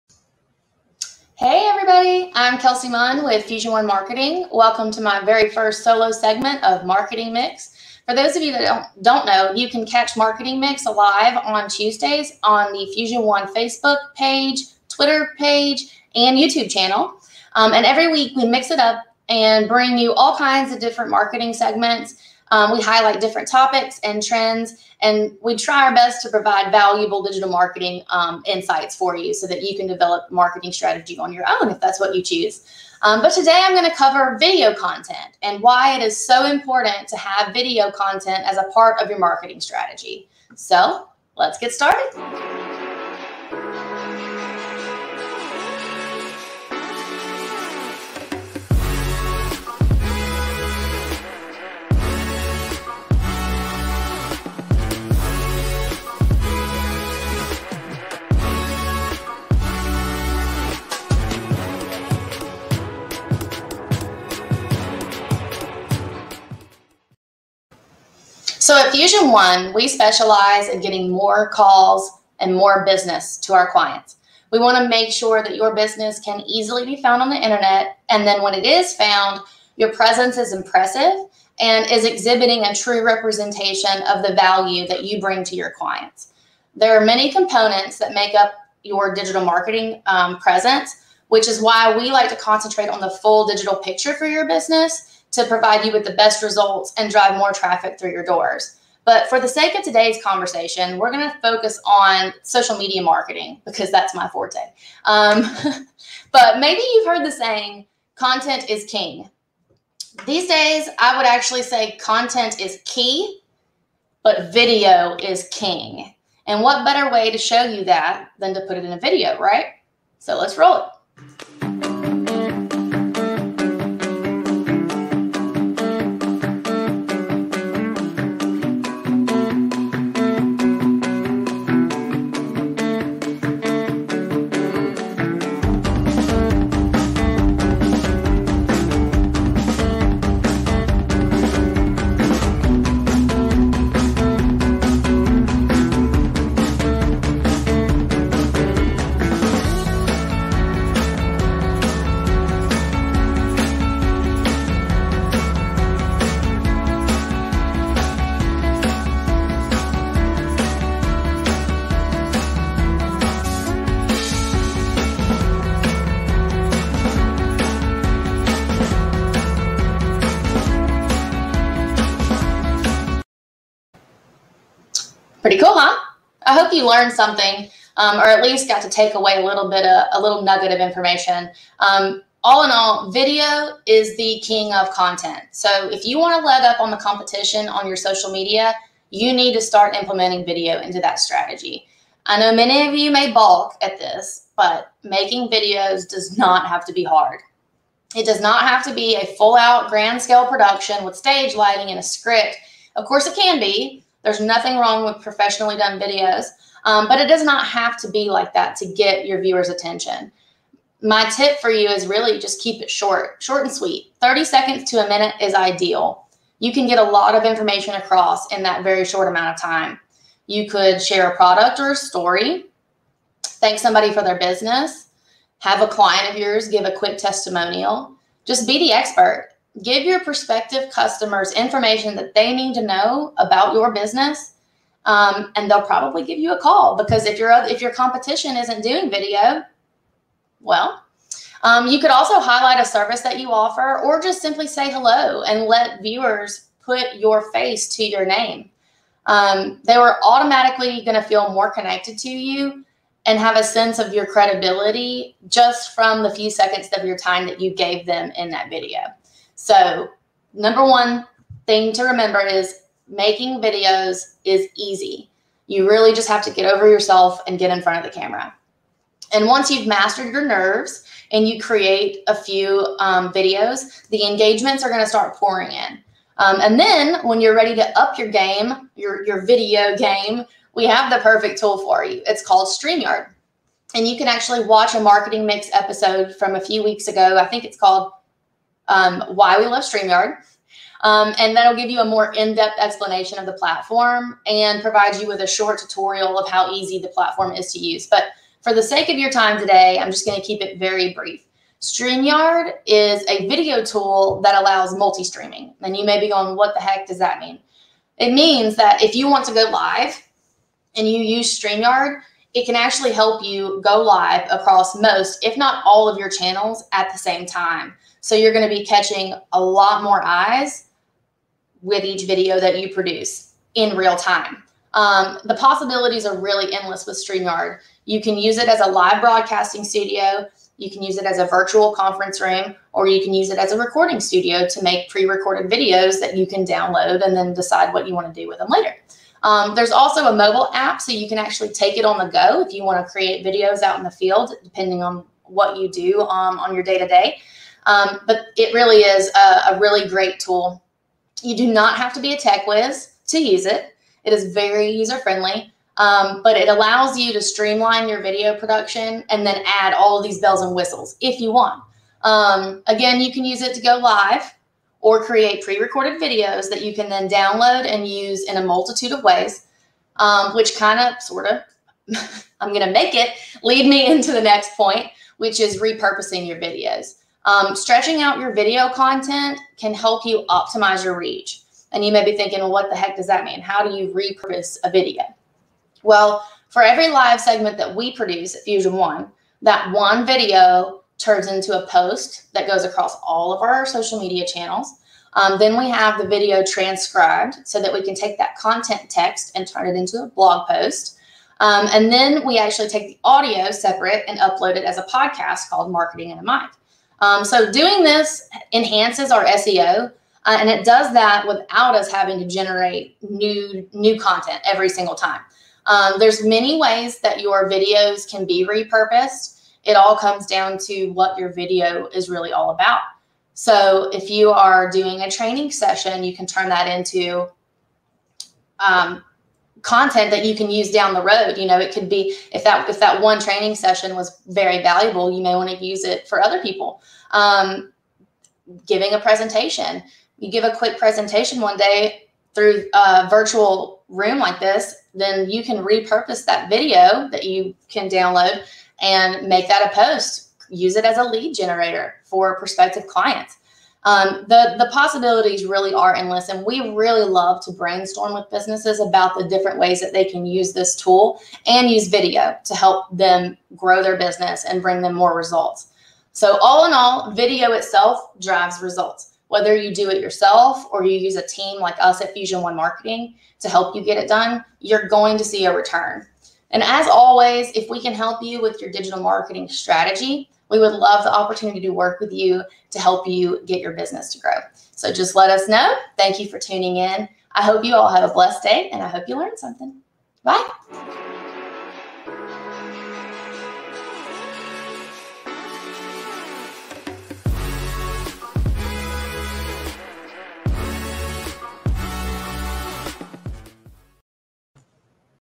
Video Content is King
Don't let your social media presence hit a plateau. Incorporating video content into your marketing strategy will take your brand to new heights!